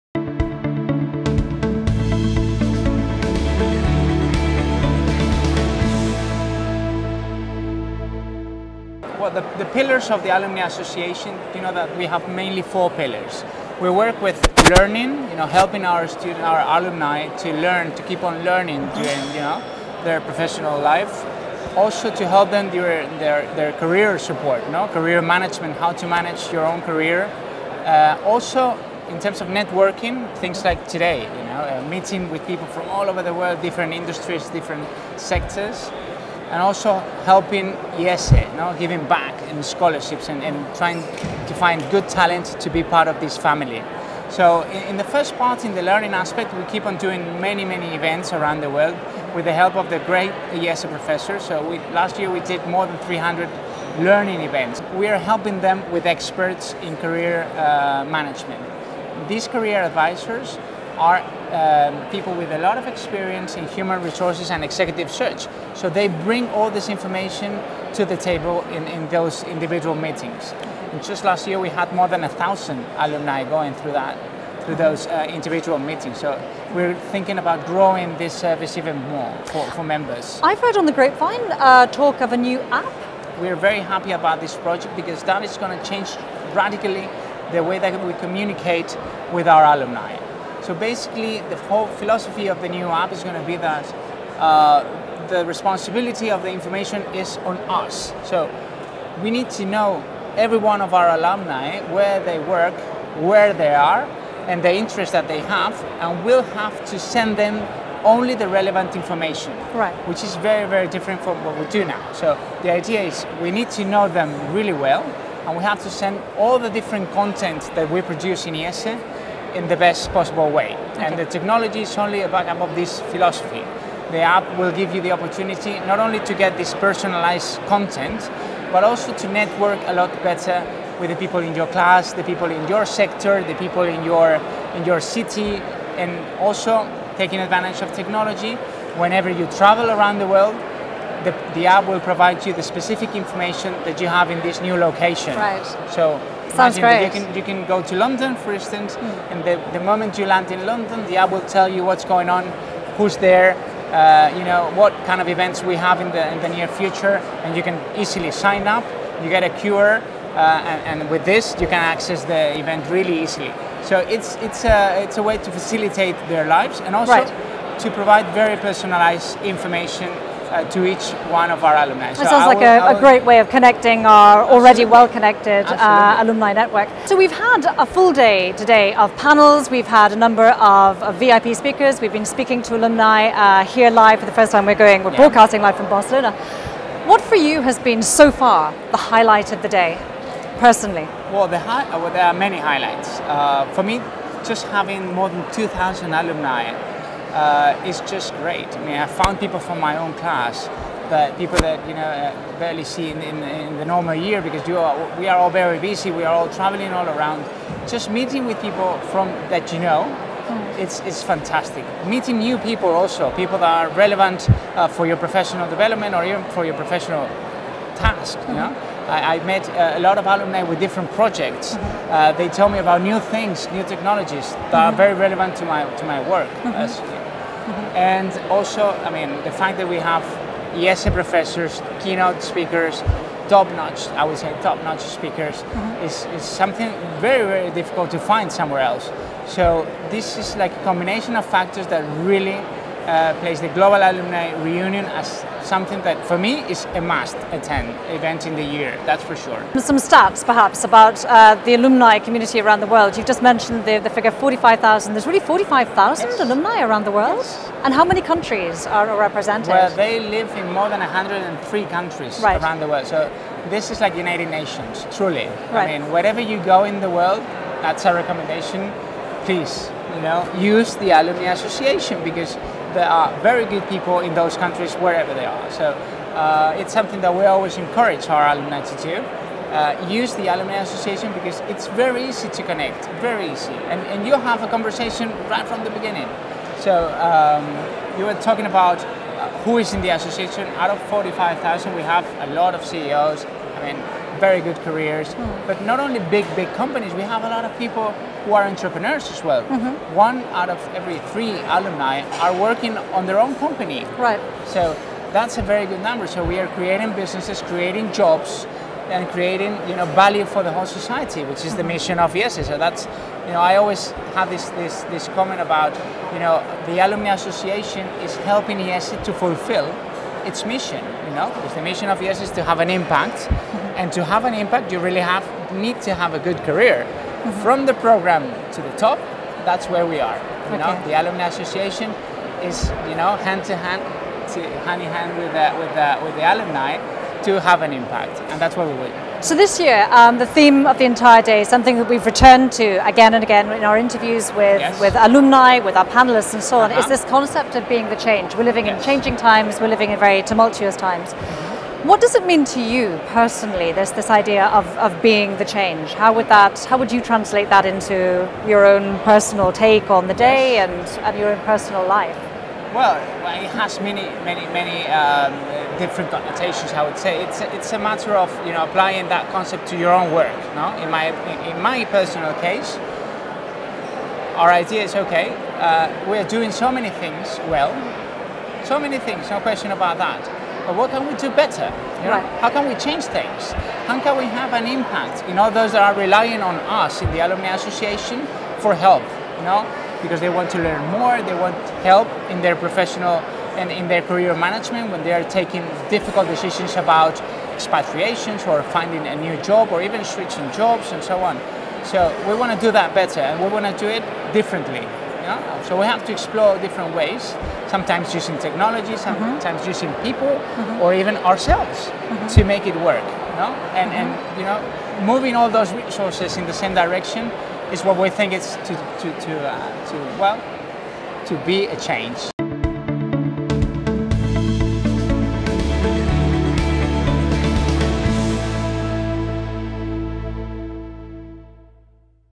Interview With Javier Muñoz. IESE Global Alumni Reunion 2016
Interview with Javier Muñoz, IESE Alumni & Institutional Development Director, during the Global Alumni Reunion 2016.